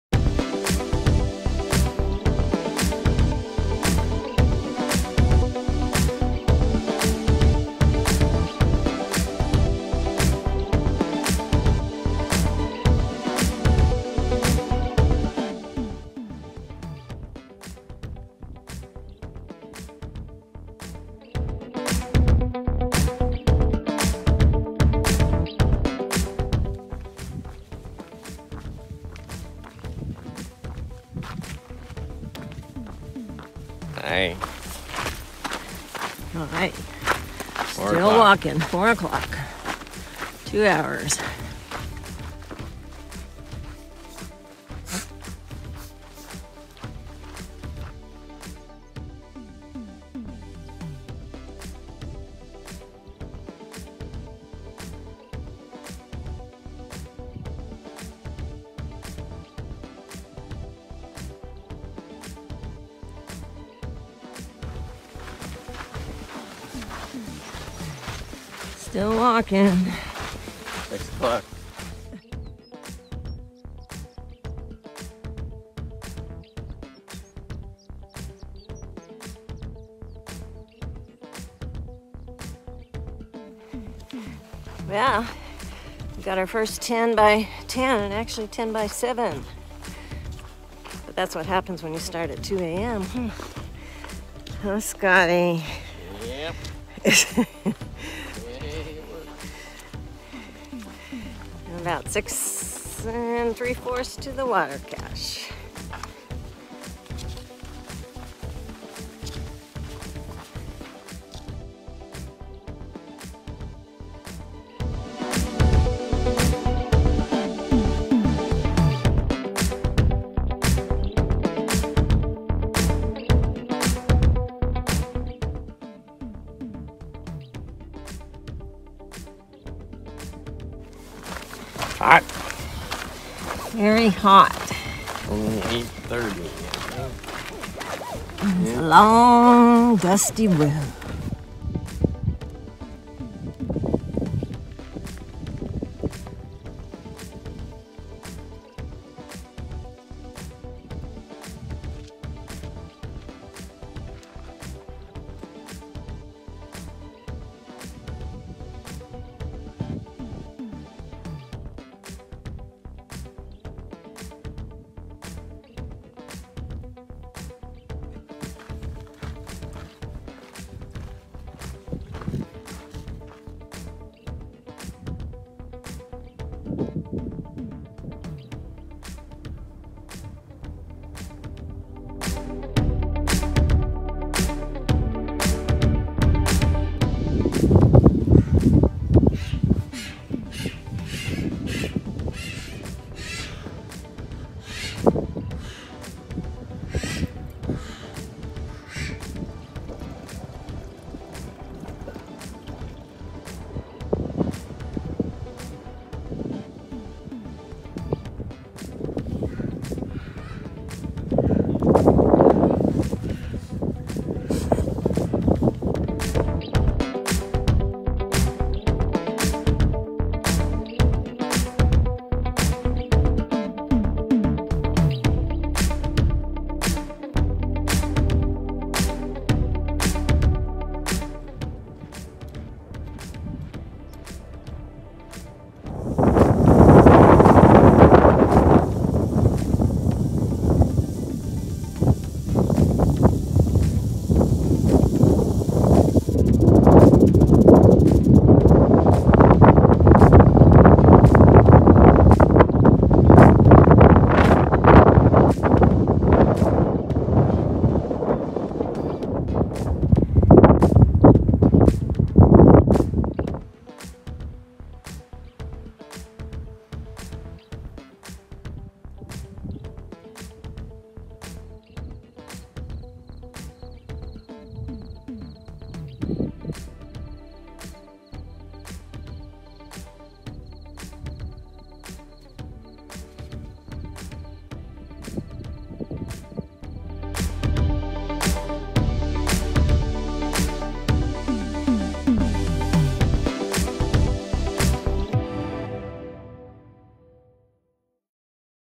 Pct 2022- Night hiking the Aqueduct
We left at 2AM to try and beat the heat for the 17 mile road walk and almost succeeded.